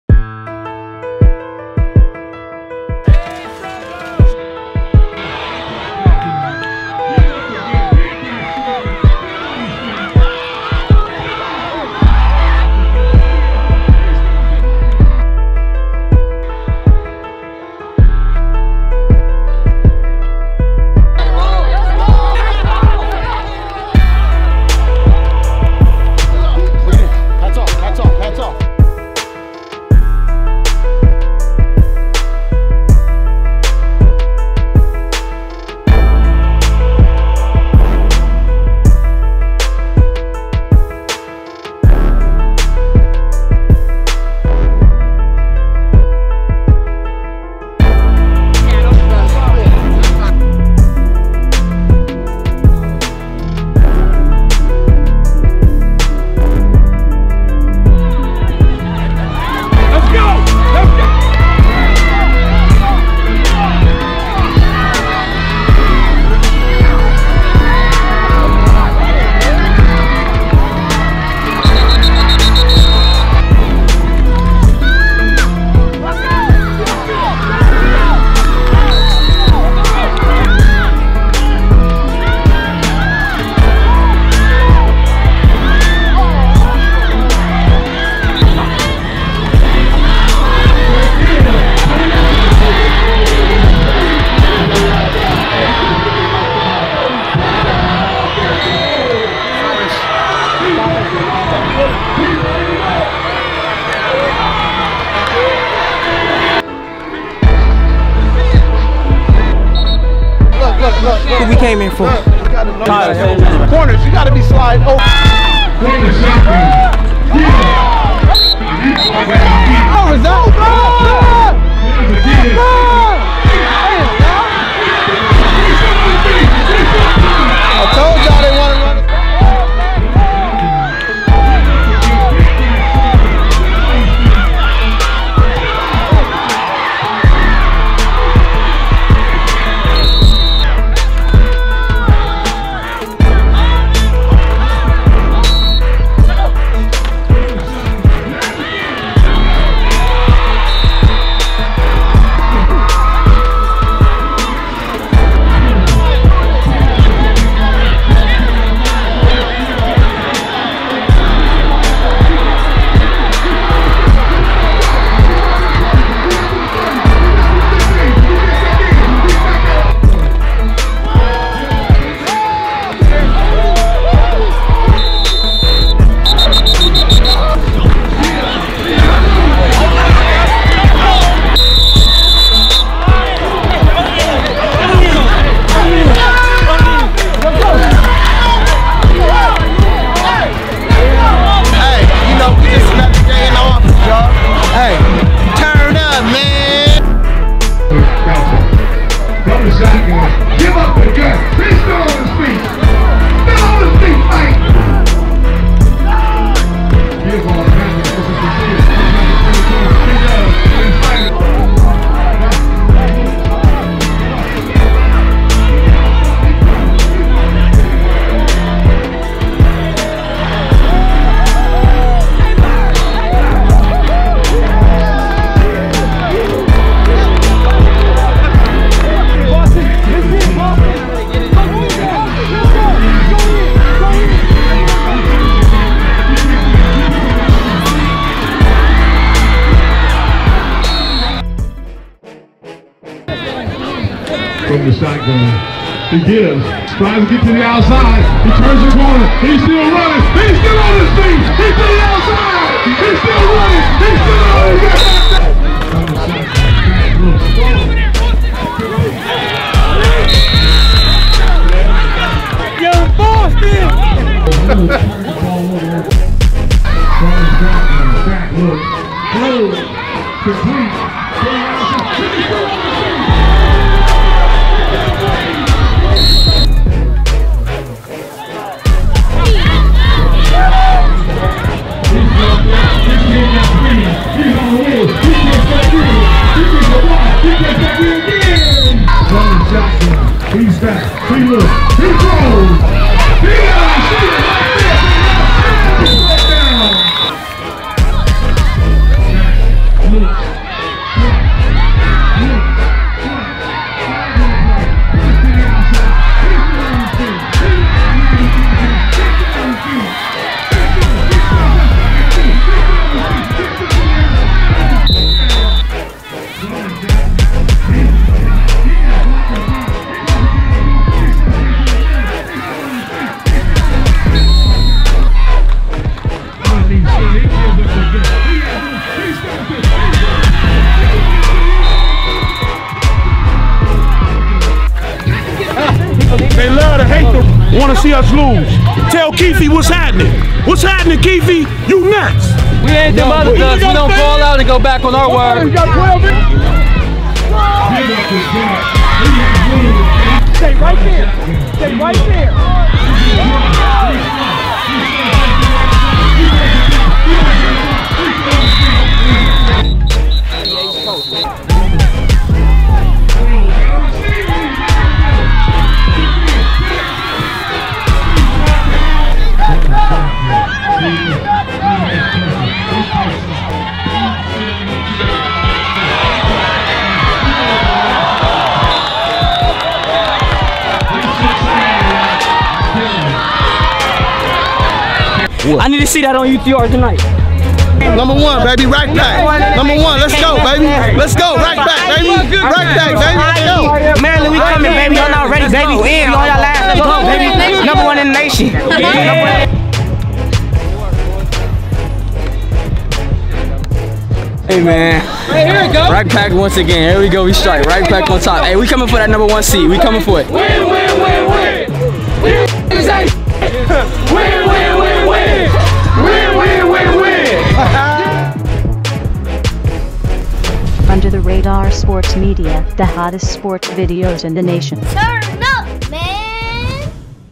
Game was LIT 🔥🔥 14U Long Beach Patriots (CA) vs South Bay 'Rac Pac' Gauchos (CA) Highlight Mix
Game was LIT 🔥🔥 14U Long Beach Patriots (CA) vs South Bay  'Rac Pac' Gauchos (CA) - UTR Youth Football Highlights - 2018